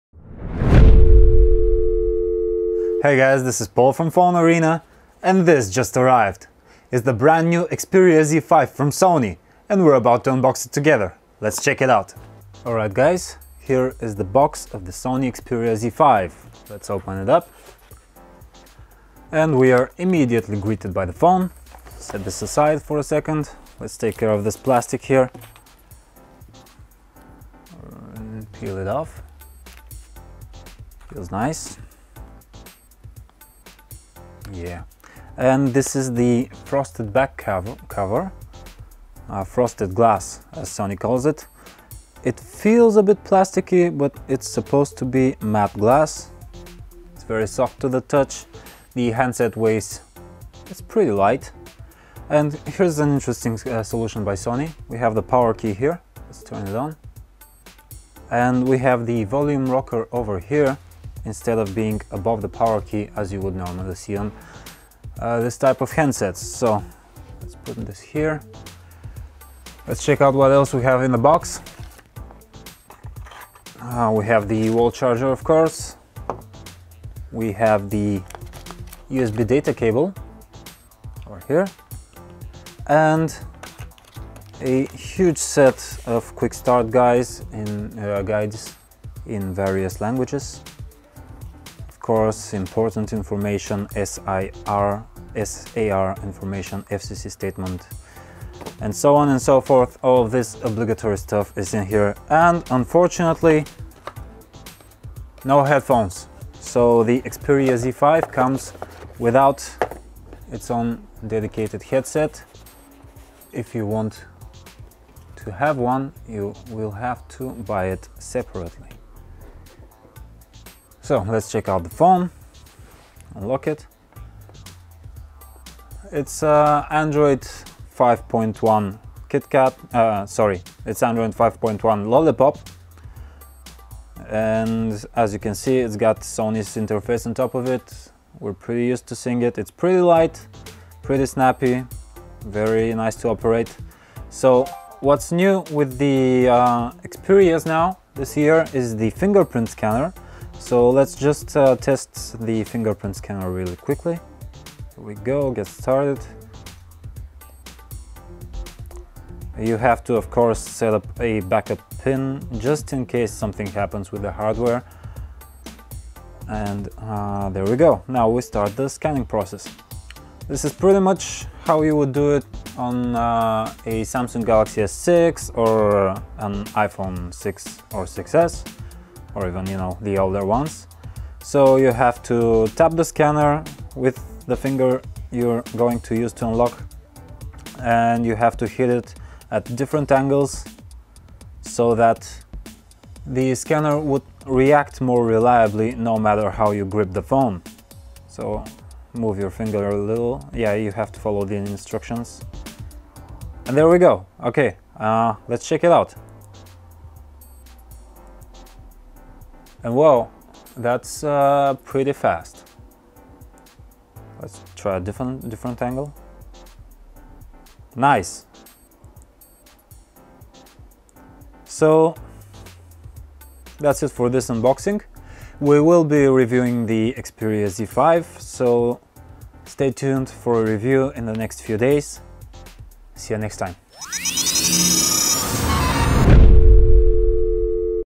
Sony Xperia Z5 Unboxing
The Xperia Z5 is finally here. It's Sony's true flagship for 2015, but it only brings minor enhancements over the previously existing Xperia Z3+. Still, if you own an older unit from Sony, you will be treated to a 64-bit processor, 3 GB of RAM, a fingerprint sensor, and a supposedly improved camera experience.